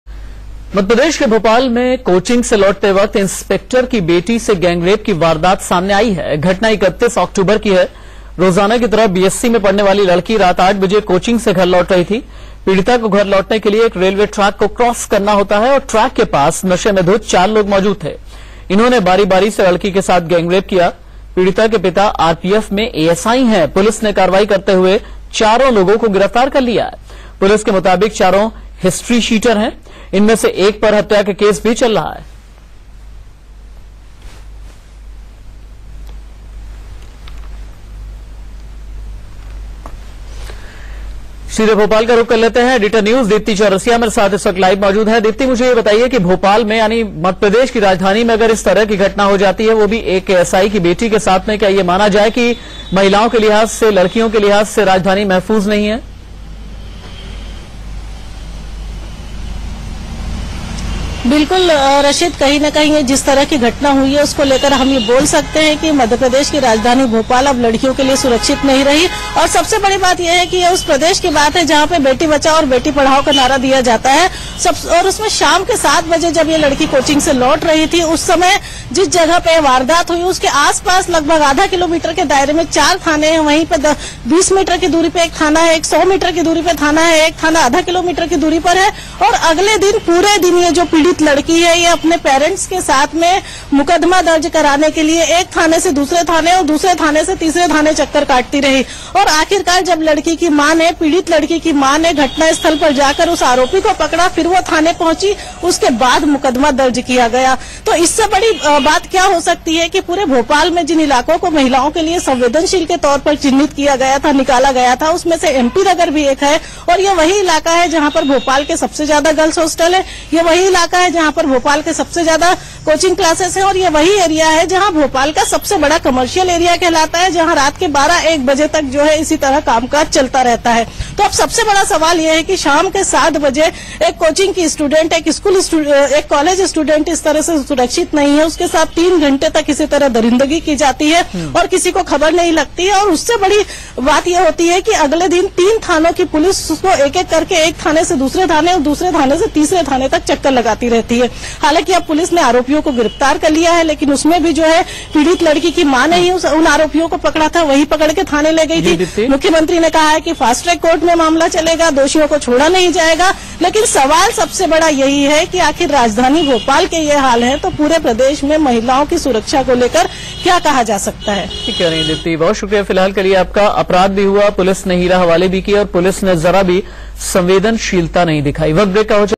भोपाल में इंस्पेक्टर की बेटी से गैंगरेप, चारों आरोपी गिरफ्तार | 19-year-old gang raped, 4 arrested
भोपाल में कोचिंग से लौटते वक्त इंस्पेक्टर की बेटी से गैंगरेप की वारदात सामने आई. घटना 31 अक्टूबर की है, रोजाना की तरह B.Sc में पढ़ने वाली लड़की रात 8 बजे कोचिंग से घर लौट रही थी. पीड़िता को घर लौटने के लिए एक रेलवे ट्रैक को क्रॉस करना होता है, ट्रैक के पास नशे में धुत 4 लोग मौजूद थे, जिन्होंने बारी-बारी से लड़की के साथ गैंगरेप किय़ा. पीड़िता के पिता RPF  में ASI हैं. पुलिस ने कार्रवाई करते हुए चारो लोगों को गिरफ्तार कर लिया है. पुलिस के मुताबिक चारो हिस्ट्रीशीटर हैं. इनमें से एक पर हत्या का भी केस चल रहा है.